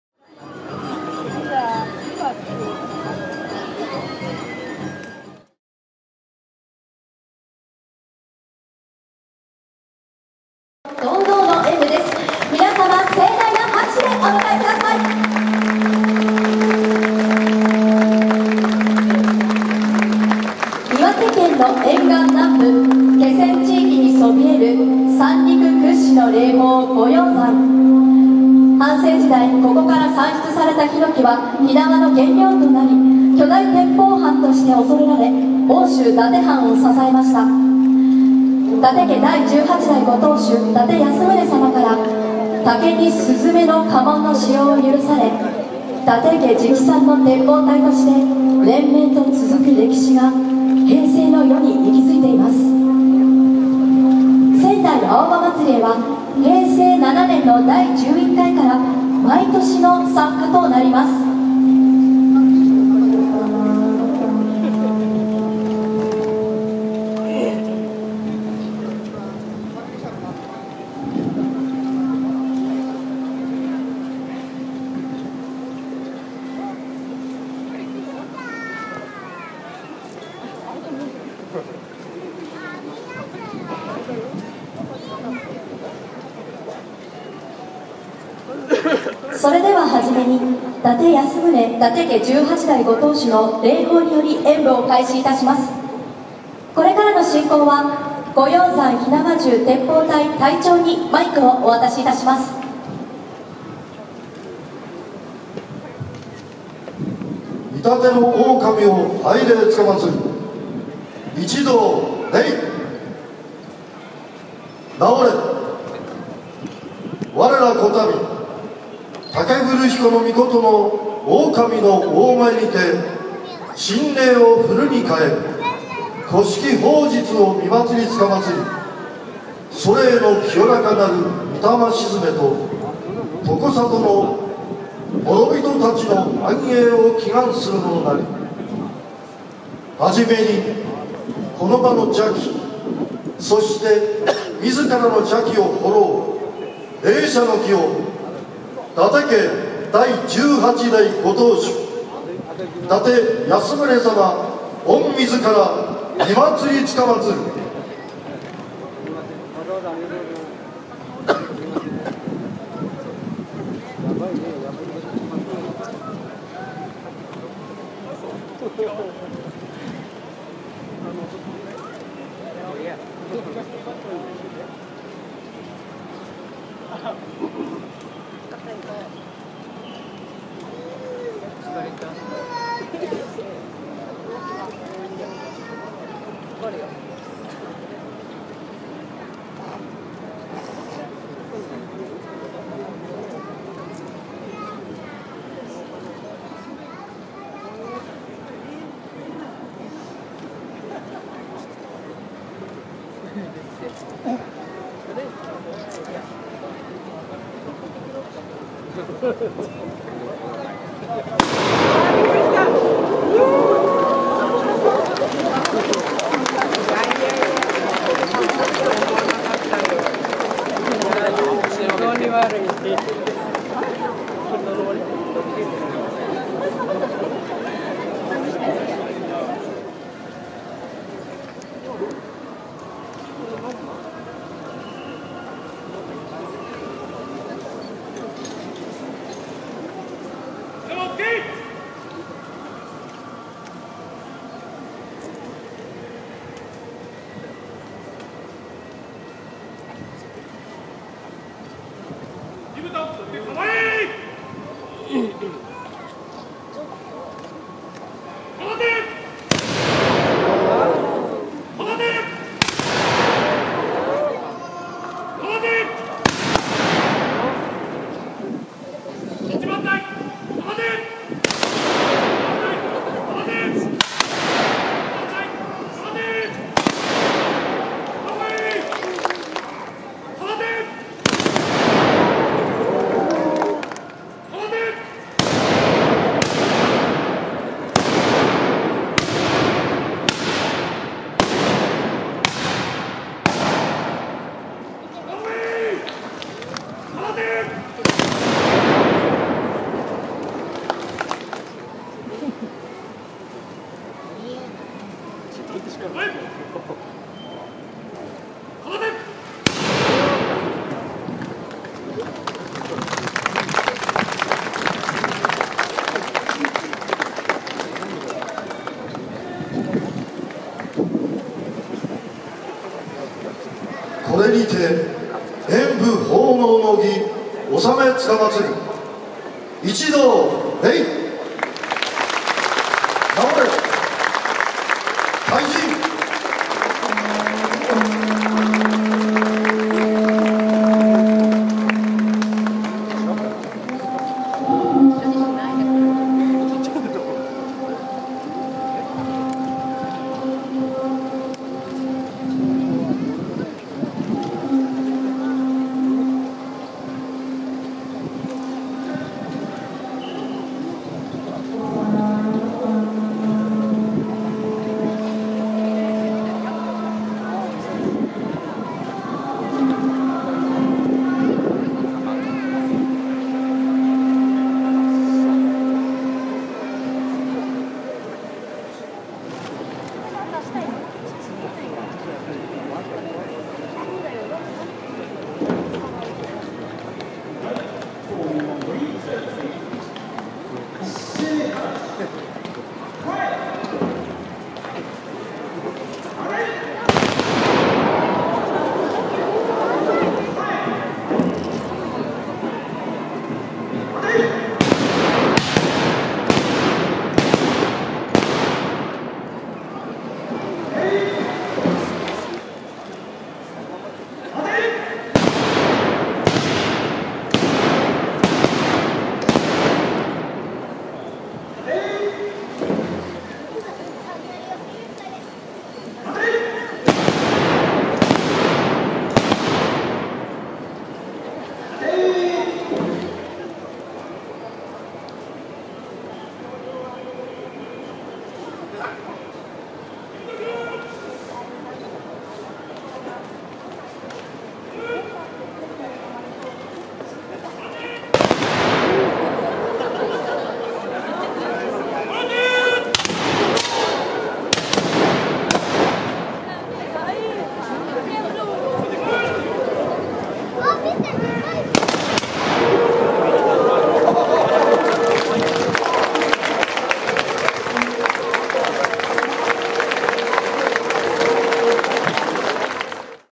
Firearms Shooting Performances, Aoba Festival 2017
Held by 3 firearms corps of former Date clan's domain:

Goyozan Firearms Patrimony of Sumita Town Iwate Pref.
Hanayama Firearms Corps of Kurihara City Miyagi Pref.
Katakura Firearms Corps of Shiroishi City Miyagi Pref.